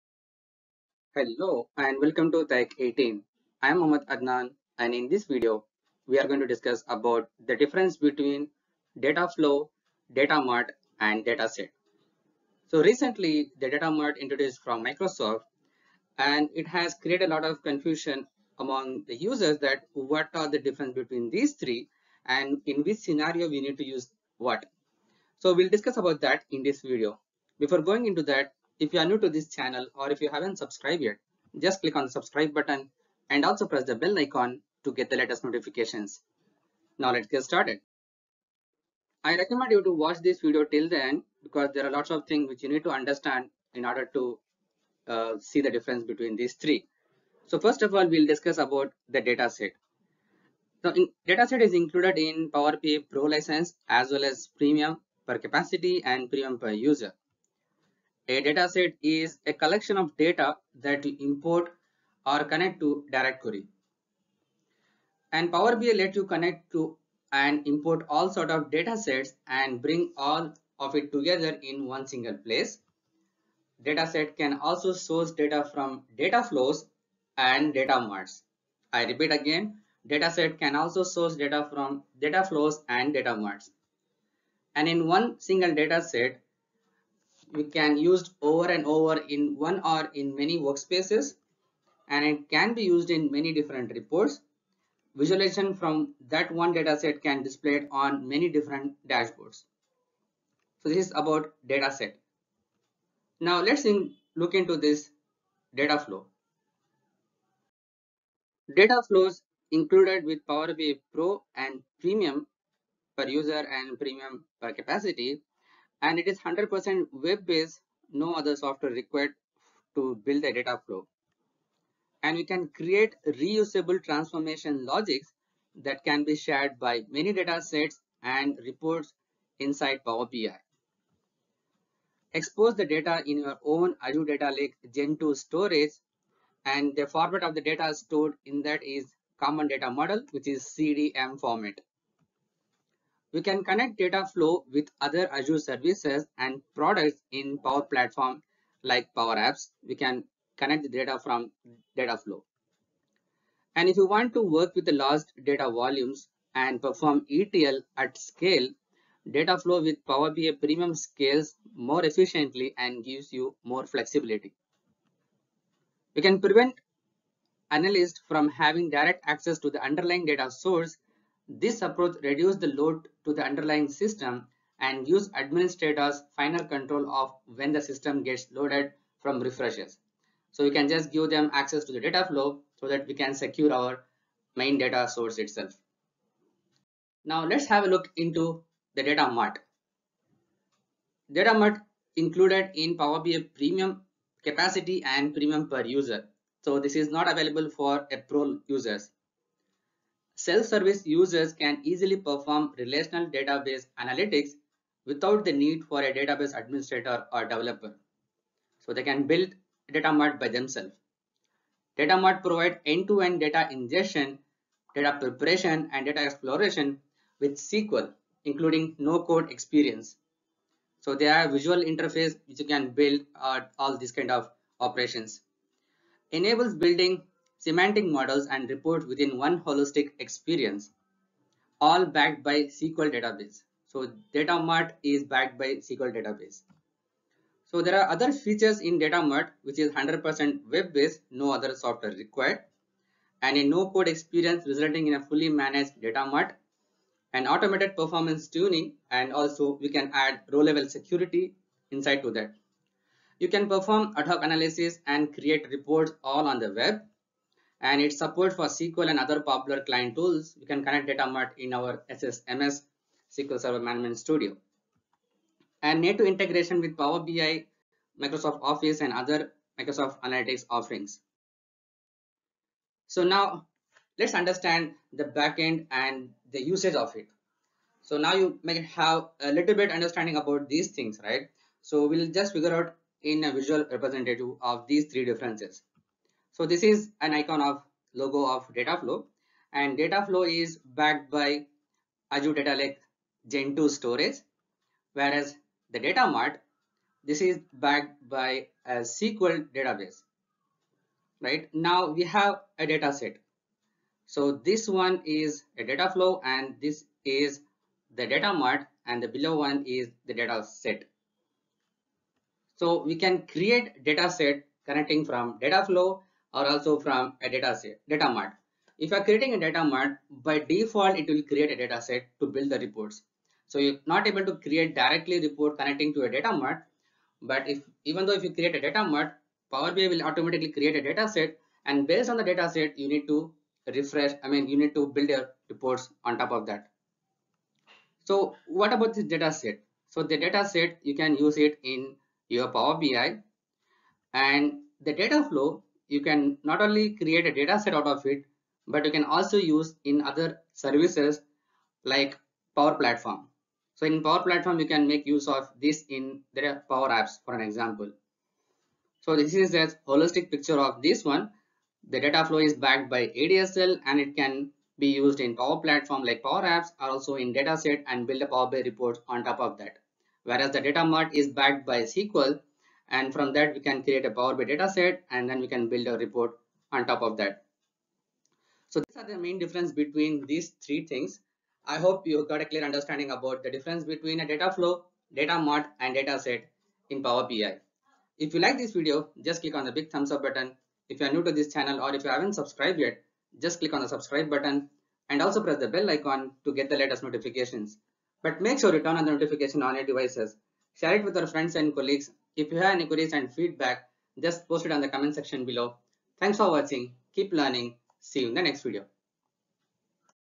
Dataflow Datamart dataset in Power BI by taik18
Learn how to use Dataflow Datamart dataset in Power BI with taik18! This step-by-step tutorial will show you how to create and utilize a Datamart dataset in Power BI for your data analysis needs. Don't miss out on this valuable tool for organizing and managing your data. Watch now!

source Microsoft

Mohammed Adnan

 I Use Techsmith Products For Video Creation,